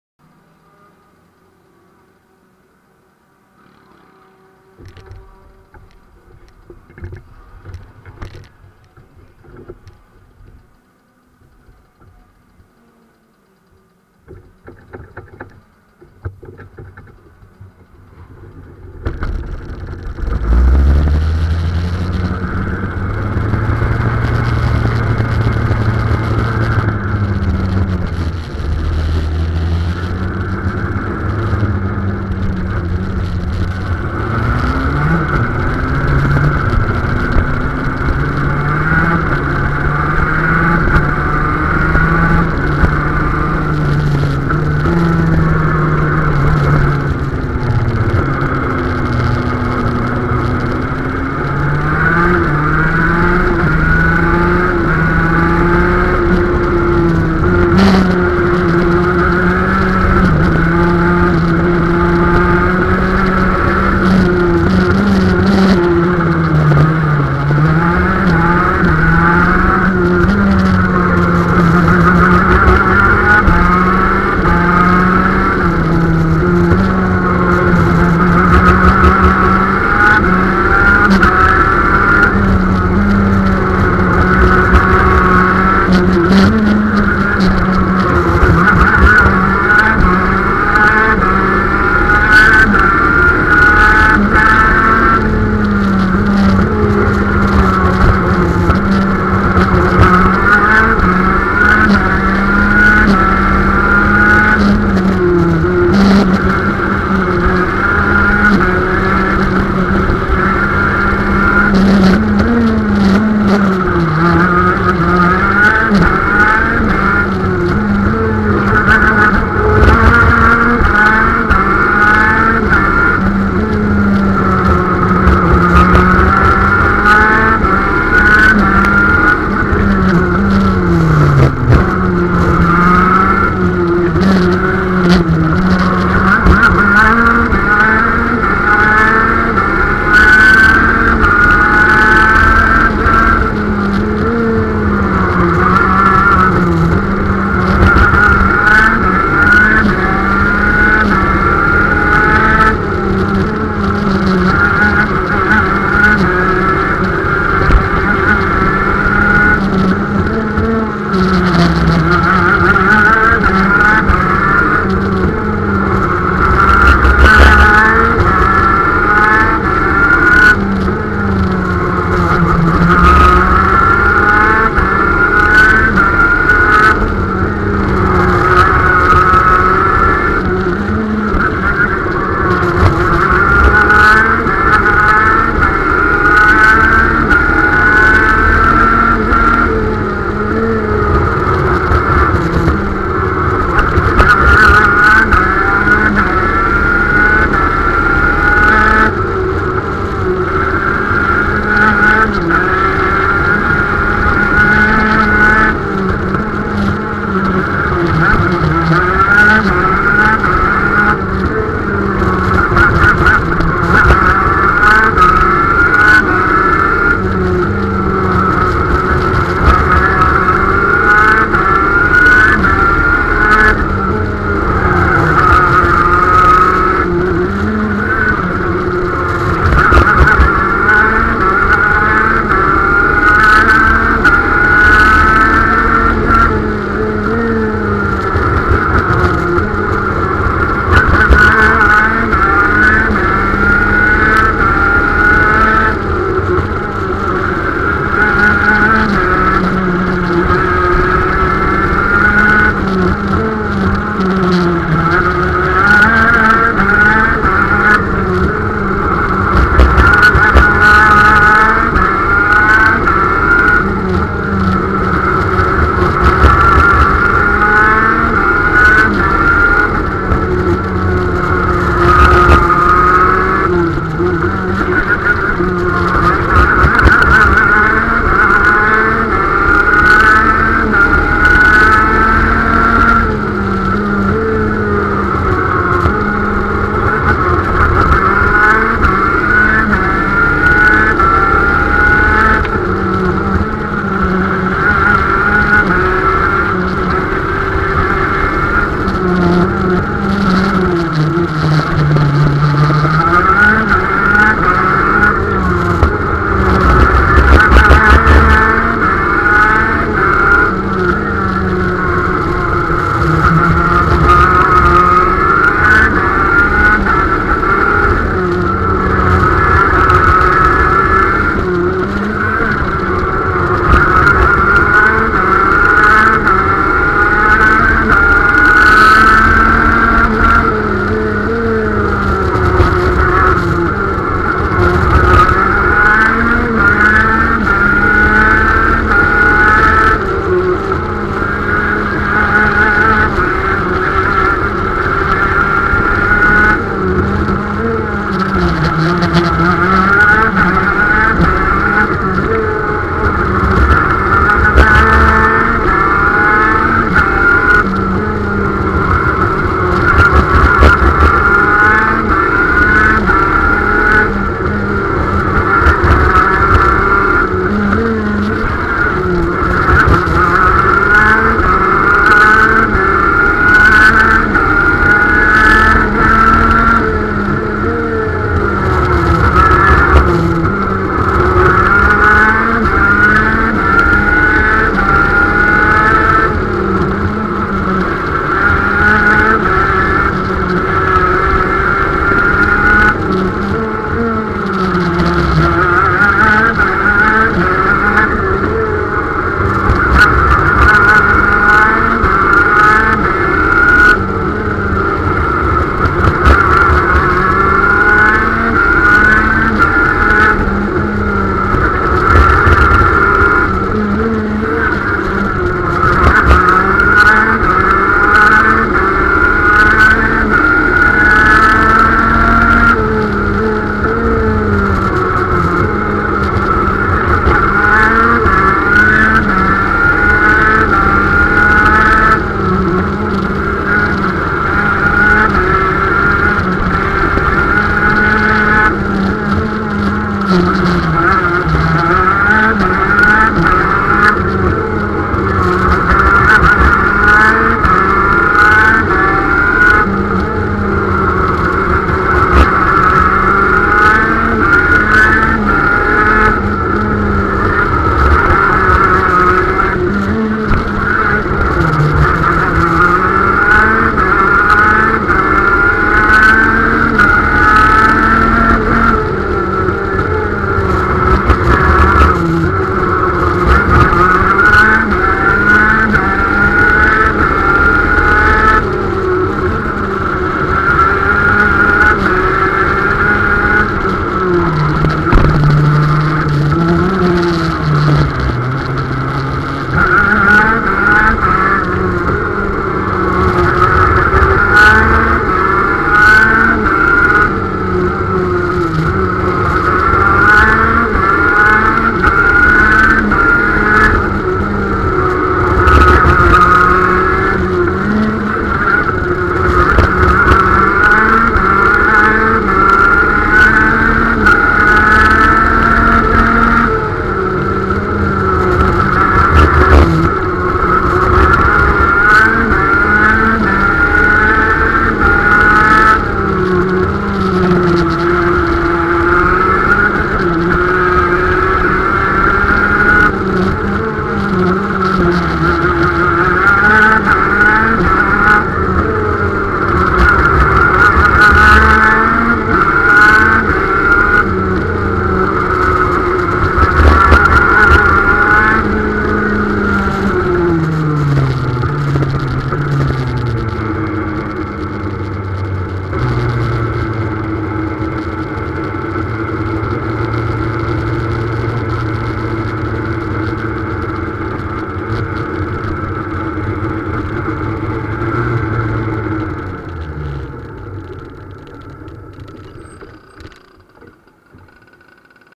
Spa KZ Kart, Mai 2017, Video 2
Erstes mal in Spa auf der Kartstrecke, Übersetzung 17/26, meine Bestzeit 45,8 Sekunden